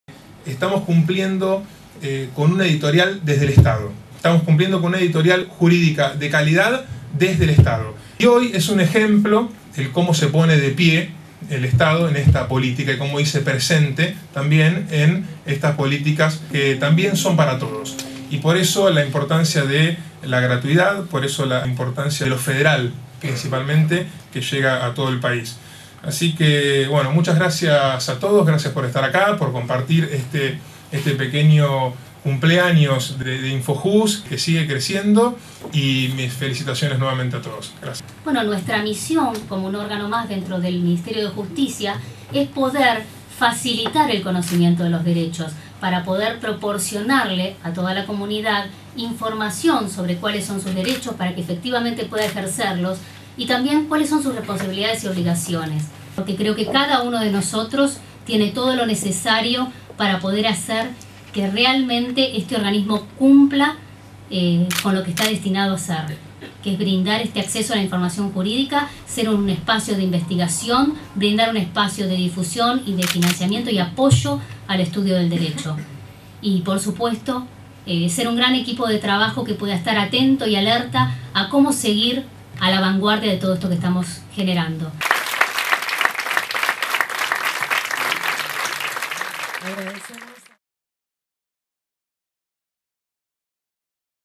Infojus cumplió dos años y lanzó un nuevo portal. 16/09/2013.
Se realizó un acto, en el salón "Emilio Mignone" de la Secretaría de Derechos Humanos, para celebrar los dos años de gestión. Lo encabezaron el Subsecretario de Relaciones con el Poder Judicial, Franco Picardi, y la Directora Nacional de Infojus, Paula Pontoriero, quienes anunciaron el lanzamiento de un nuevo buscador que permite un acceso más ágil a la base de datos que contiene una extensa y actualizada biblioteca de jurisprudencia, doctrina y leyes nacionales y provinciales.